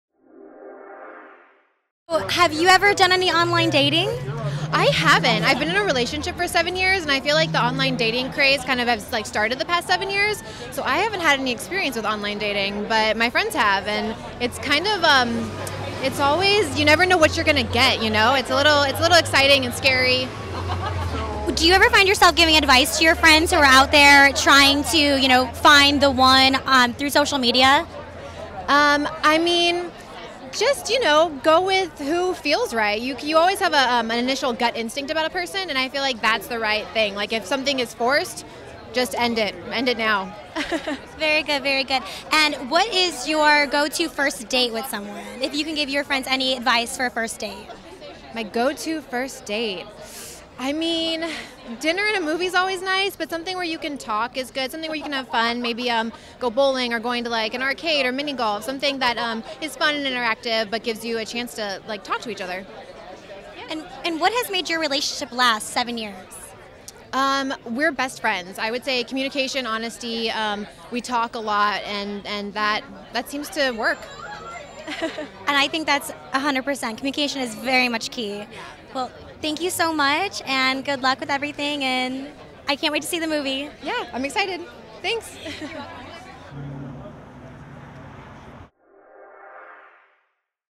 Najarra Townsend Interview WHAT NOW World Premiere Red Carpet
Exclusive interview with actress Najarra Townsend at the special screening red carpet premiere of WHAT NOW movie at Laemmle's Music Hall in Los Angeles, Ca March 10th, 2015 - Interview by Noel Kensington